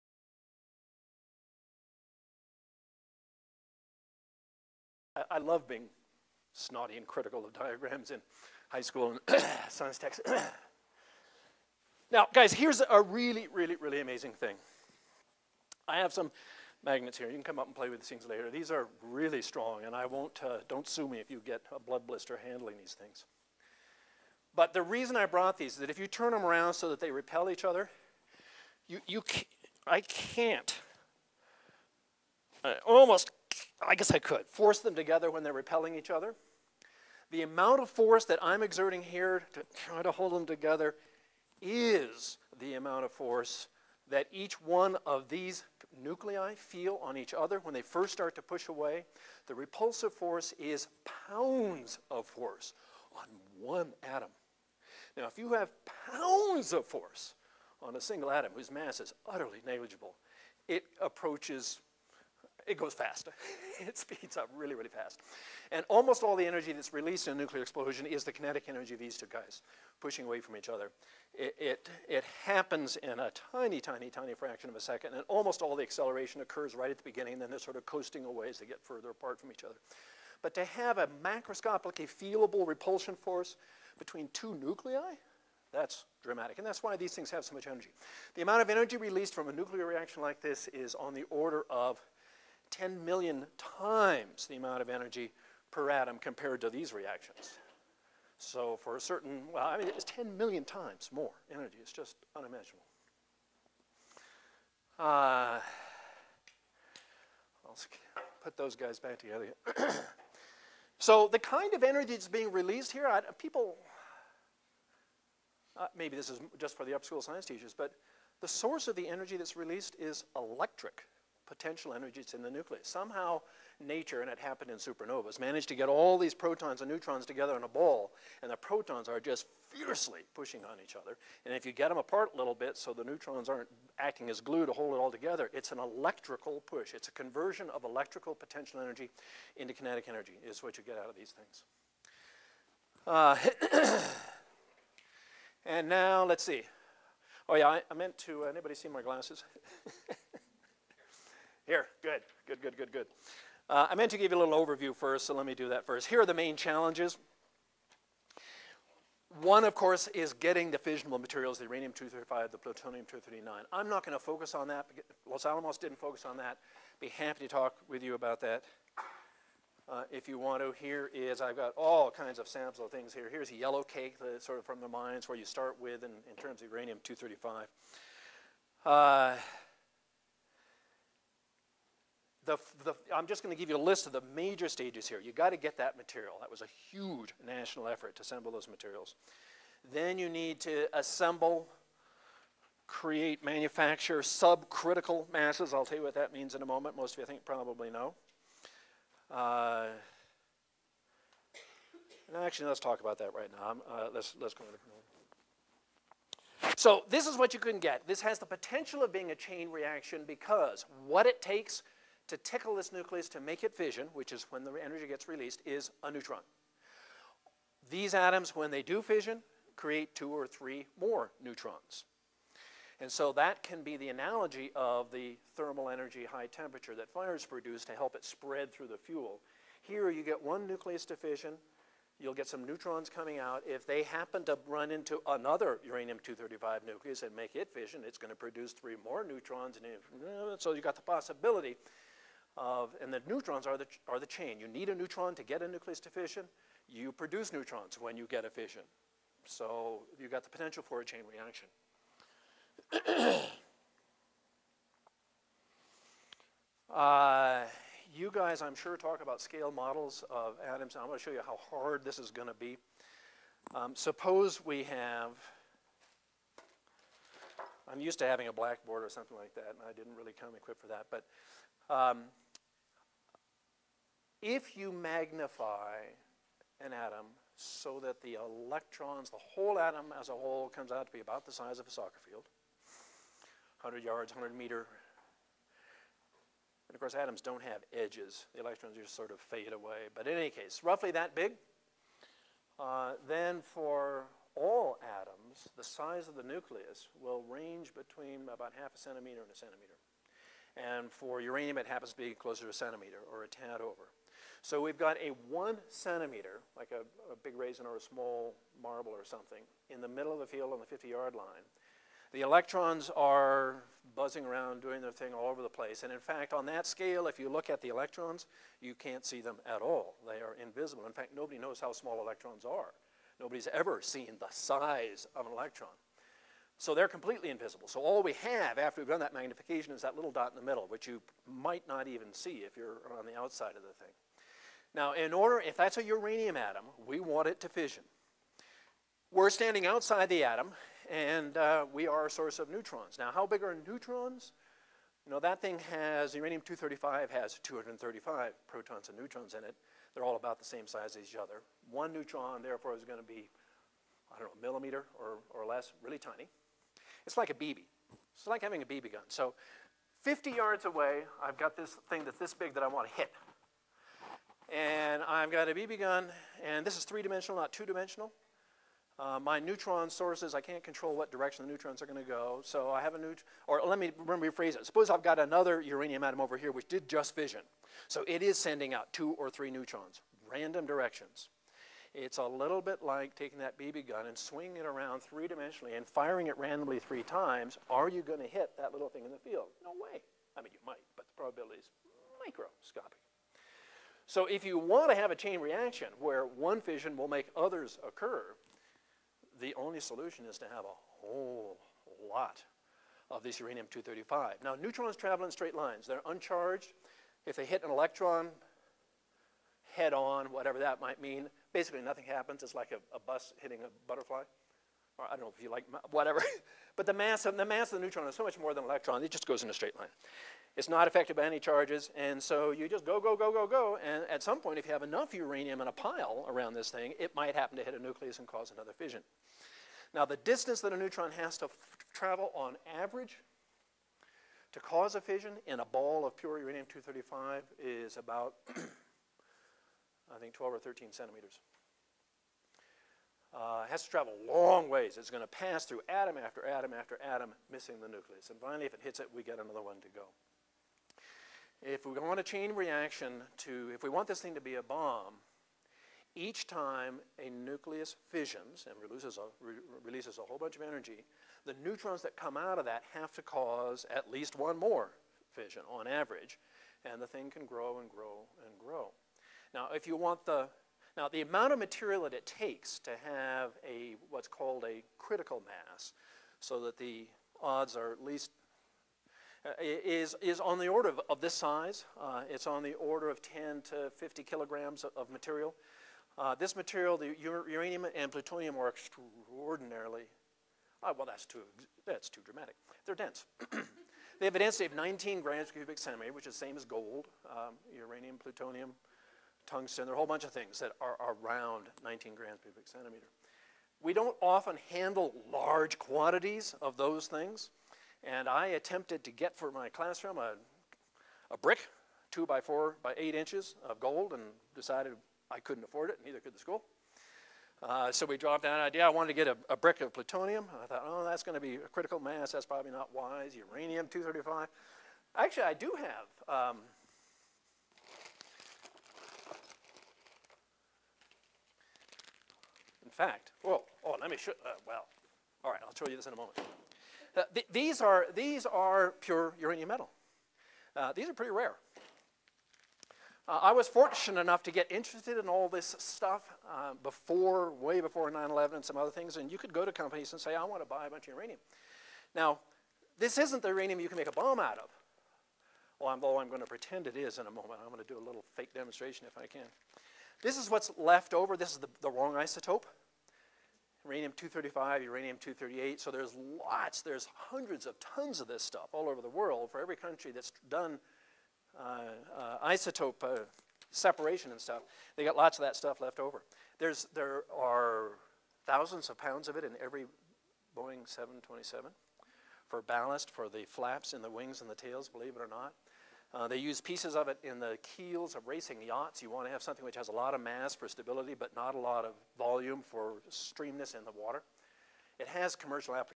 Jay Shelton - Geography and Science of the Atomic Bomb (Part 2 of 7)
Jay Shelton discusses the Geography and Science of the Atomic Bomb at the Atomic Heritage Foundation's 2009 Teachers Workshop on the Manhattan Project in New Mexico.